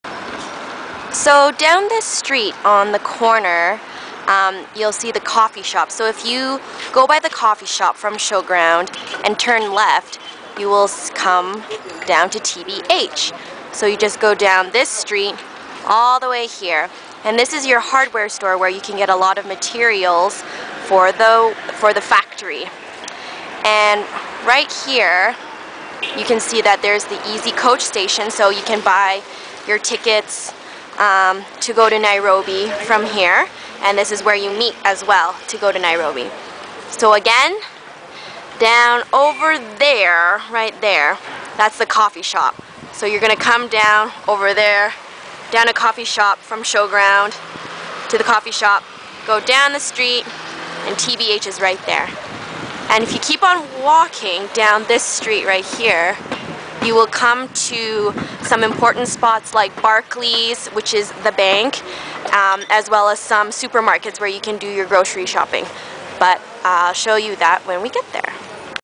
Getting to Know Kiminini & Kitale: Part 7
Serena Cheung stops at the showground to show us where to get to the Coffee Shop and Easy Coach station.